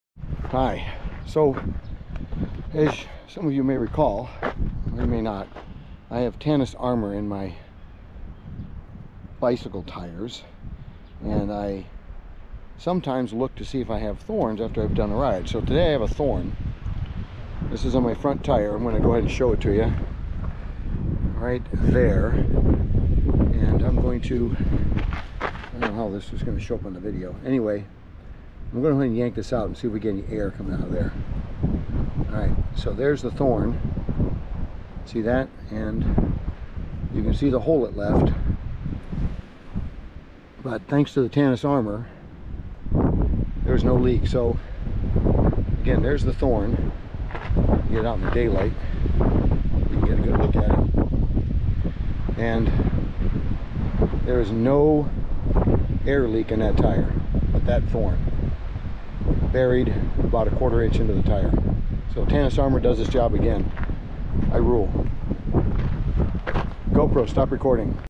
Dave Reviews Tannus Armour Episode Three
Today I picked up a thorn at Scott County Park in Iowa.  Let's see how the Tannus Armour in my tires performed.  The Armour has been in my tires for about four years now.